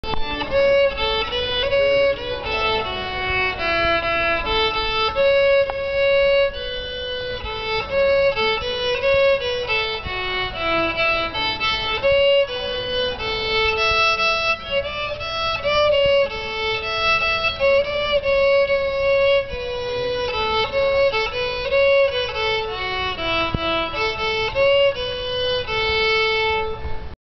December 2007 - Noah plays at grandma's funeral
Noah was brave enough to play one of my favorite songs at my mother's funeral last year.  I will always thank him for that.